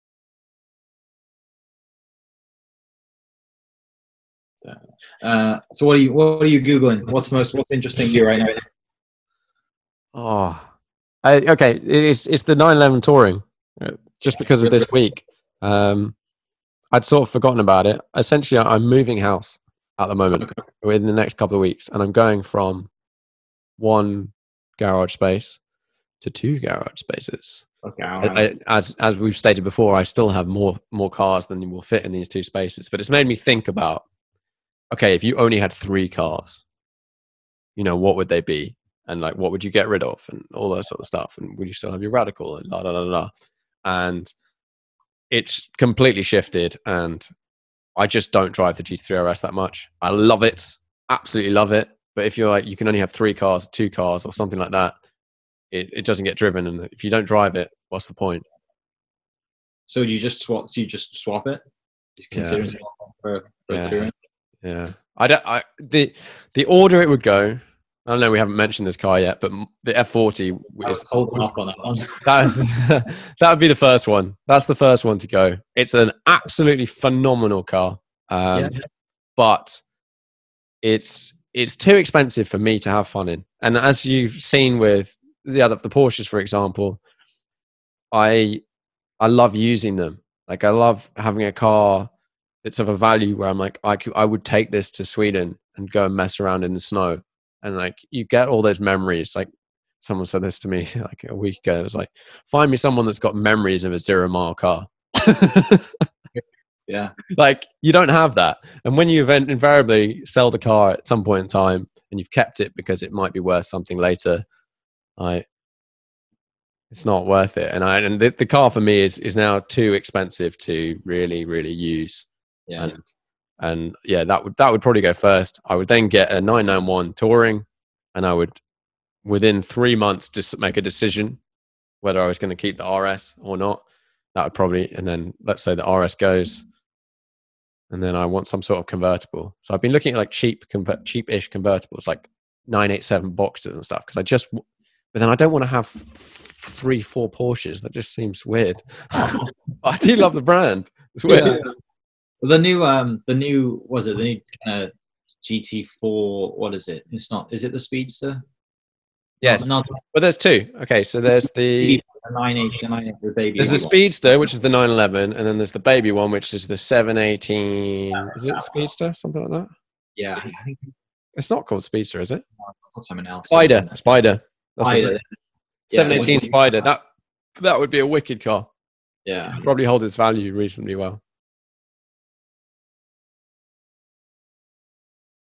Sam Moores - I've Got My Eye On A New Porsche
Sam Moores comes on the podcast and talks about his future ideas on cars for the garage. Will the RS stay? What's coming?

Follow Sam @sammooresphoto
Follow Us @randomcarguys